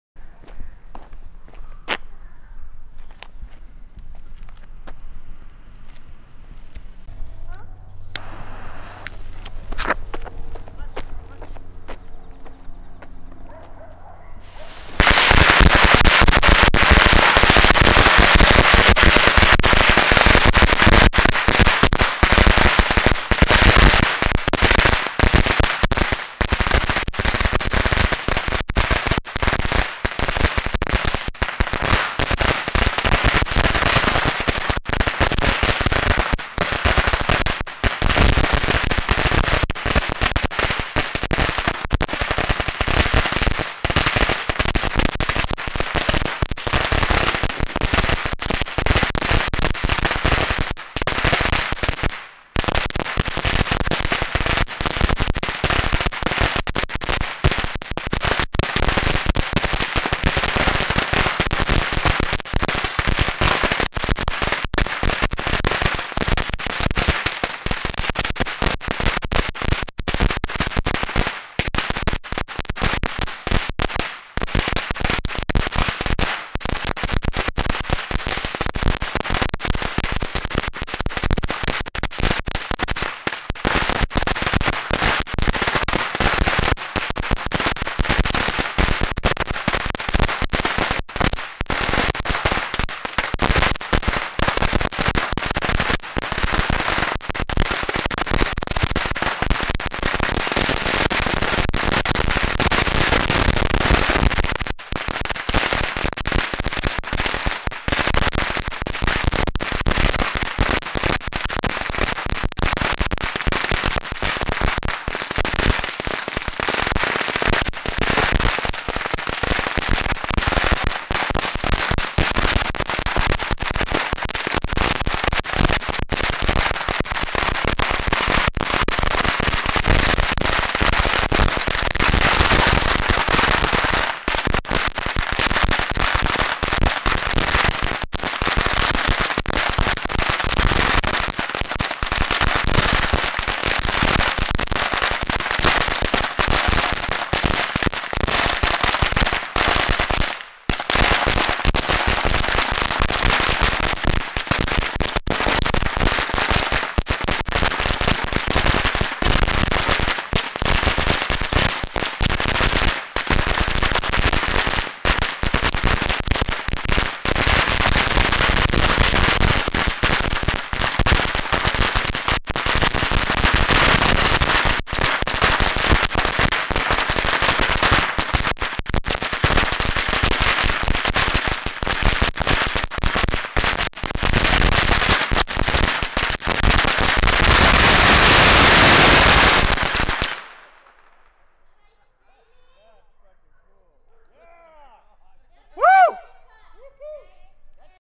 7 Thousand Black Cat Strip
3 Bricks of Black Cats and one Black Cat Firecracker Bomb with salute mixed in.